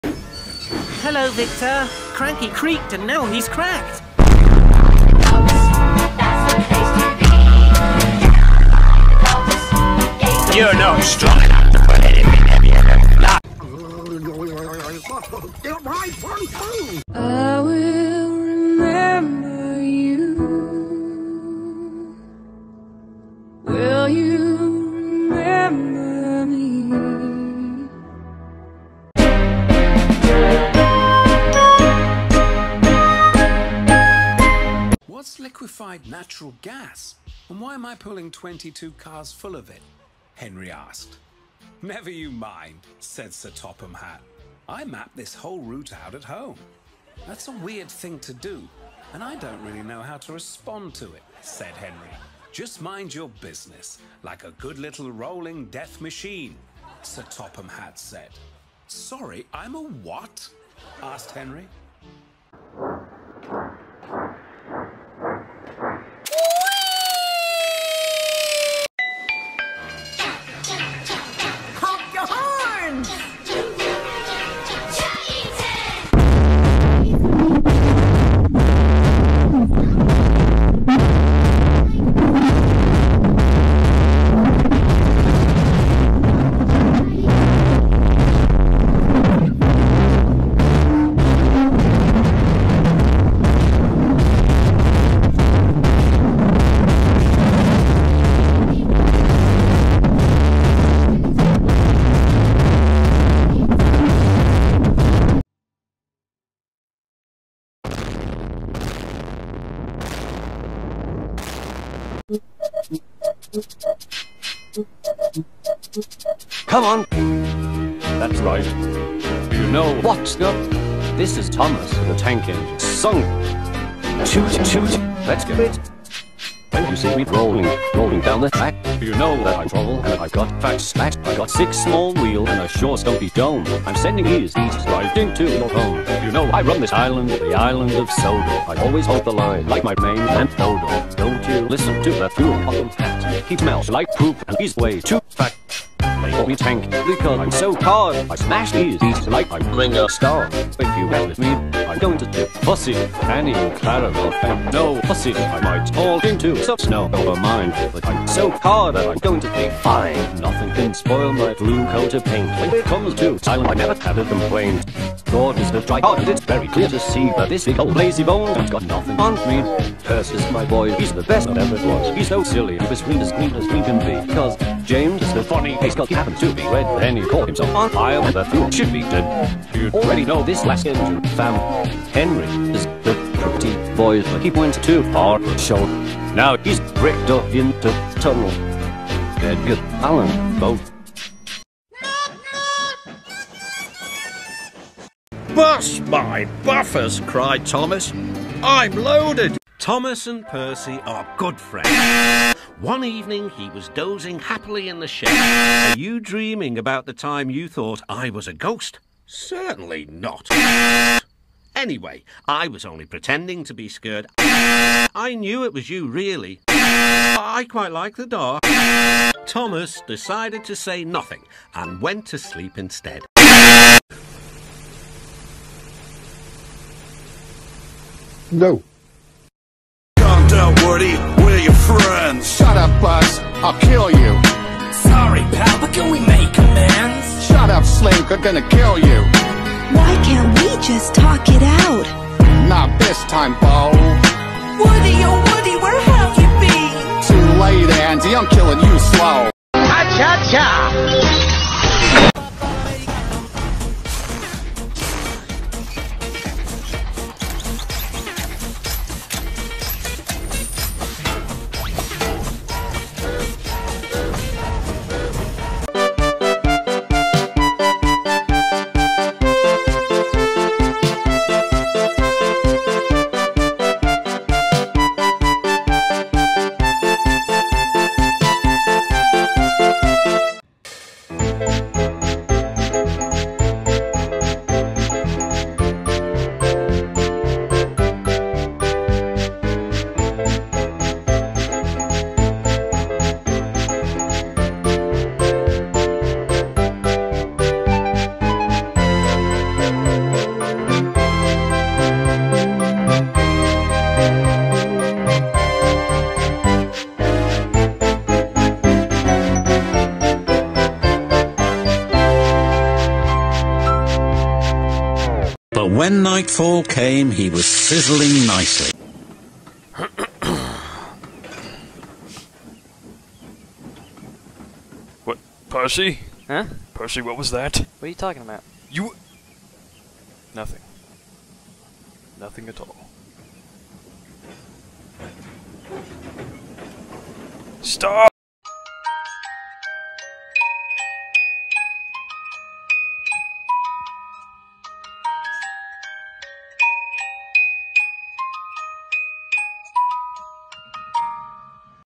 Thomas Discord memes that will cancel AEG
Twice as long and two times funnier than the last one.

Btw these memes were submitted in my discord server so don’t get mad if you have seen these on YouTube.